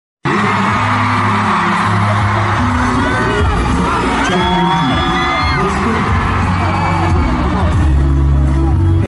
Jungkook reaction to IU's name MAMA 2017
Clearly chsnting IU~ IU~
"All Credits to the right owner. this is not for personal use or money making."
check us below for more (photo, edits and etc.)